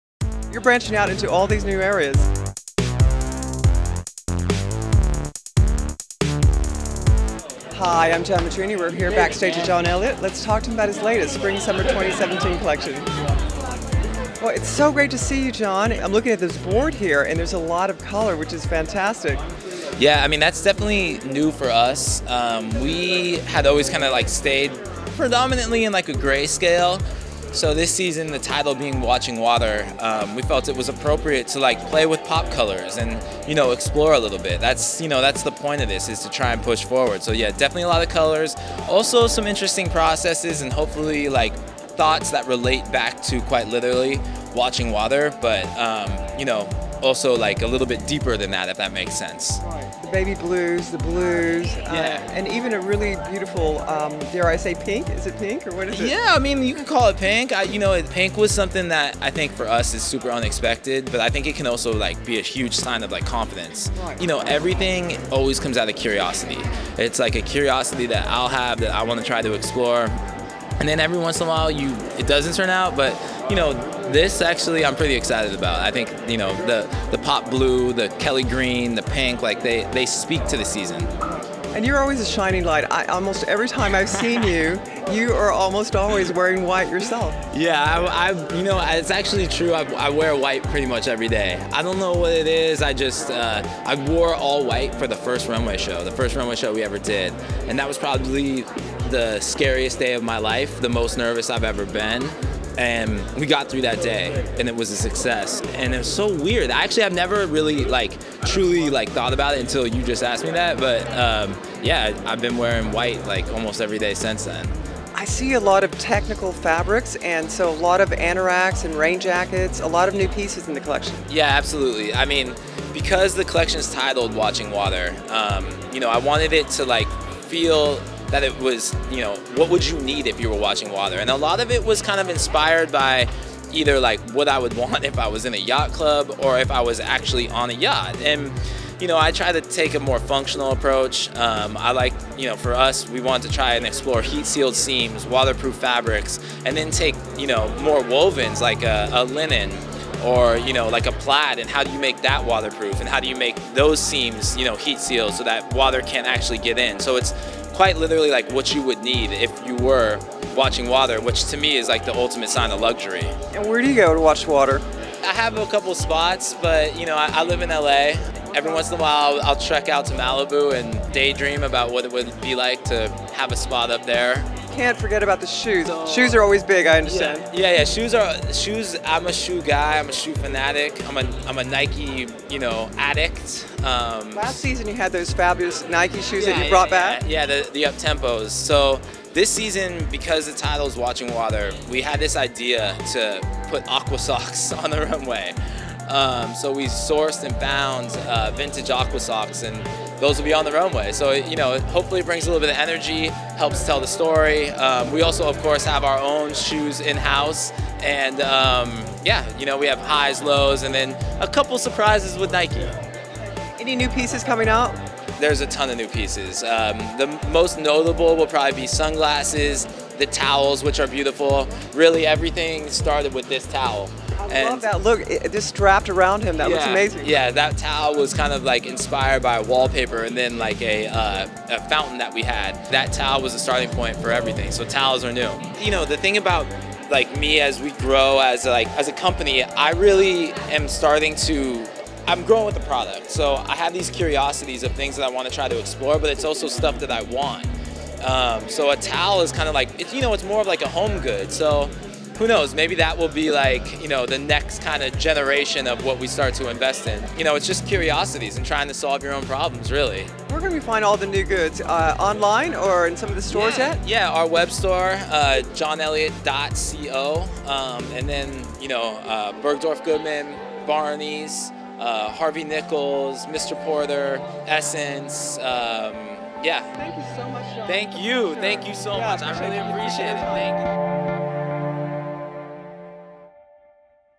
John Elliott Spring / Summer 2017 Behind The Scenes Exclusive Interview | Global Fashion News
John Elliott Spring / Summer 2017 Menswear Interview and Ready-To-Wear Collection by designer John Elliott

Fashion Editor/Stylist: Eugene Tong
Makeup Artist: Colleen Runne
Casting Director: David Milosevich
Models: Adonis Bosso, Alexis Ordonez, Aly N'Diaye, Austin Weinreb, Daniel McSweeney, Daniel Oh, Darius Johnson, David Agbodji, Dean Zruya, Dillon Westbrock, Erin Mommsen, Fernando Cabral, Geron McKinley, Hamid Onifade, Hunter Taylor-Martinez, Jackson Hale, Jeremy Matos, John Kolic, Jonathan Bellini, Kendall Harrison, Louis Mayhew, Max Masters, Michael Lockley, Nathaniel Dam, Noma Han, Roberto Sipos, Rockwell Harwood, Sheani Gist, Sung Jin Park, Sup Park, Wonjung Jo, Youssouf Bamba
Fashion Editor/Stylist: